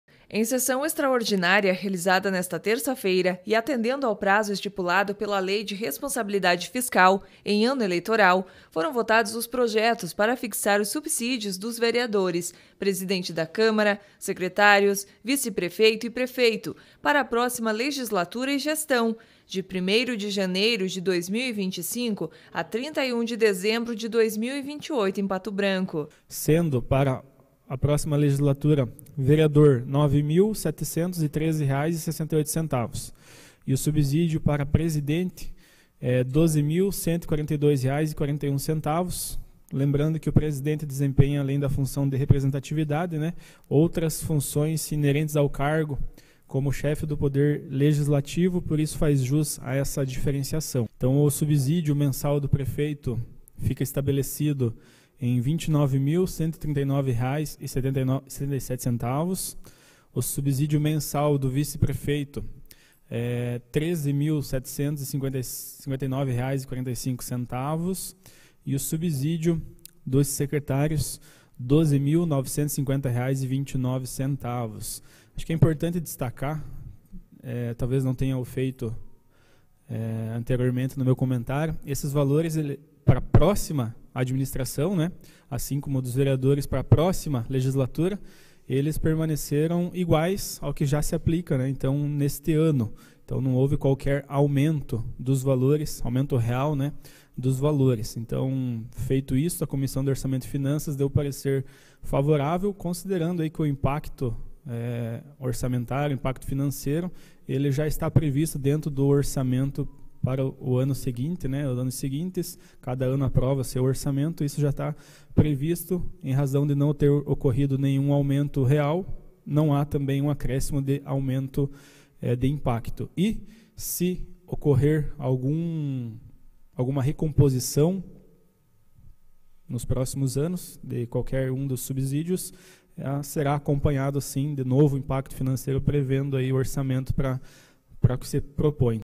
Vereadores aprovam subsídios para a legislatura e gestão 2025 a 2028, de Pato Branco
Gestão Pública: Foi aprovado em primeira votação, o projeto que fixa a remuneração da próxima legislatura e gestão, de Pato Branco.